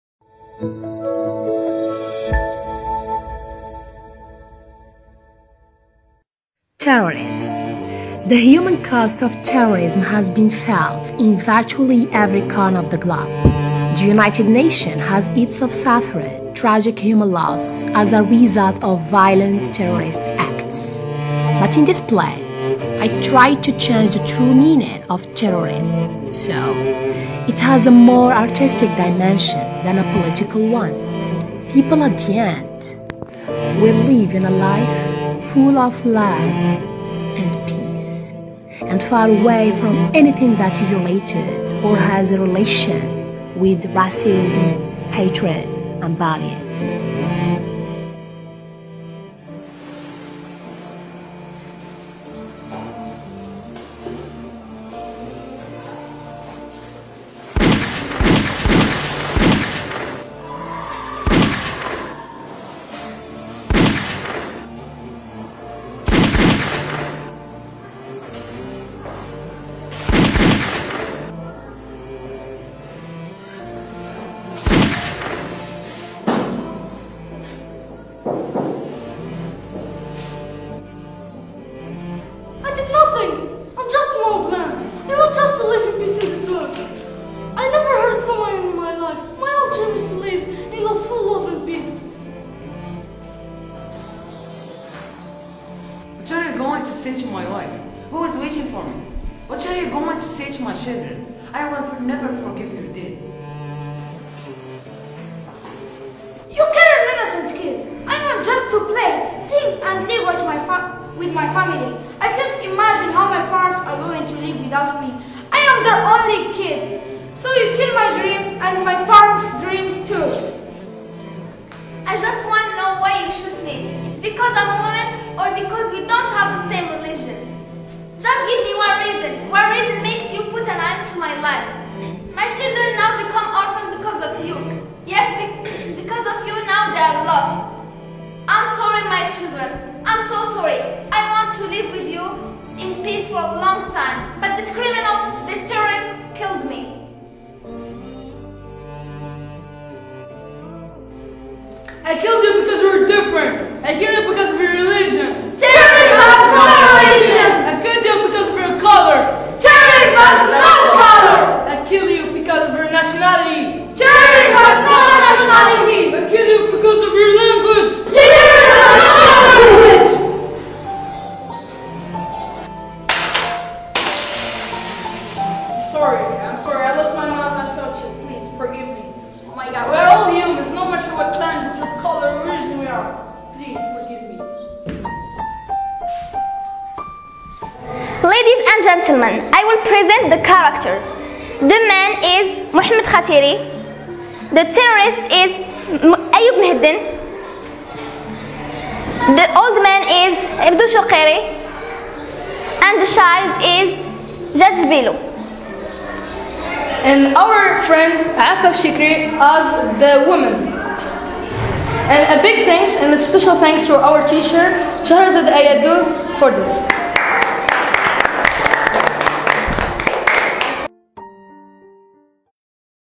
Travail théâtral en anglais: La lutte contre le terrorisme en tant que acte anti-humain
En vidéo un travail théâtral en anglais portant sur la lutte contre le terrorisme en tant que acte anti-humain !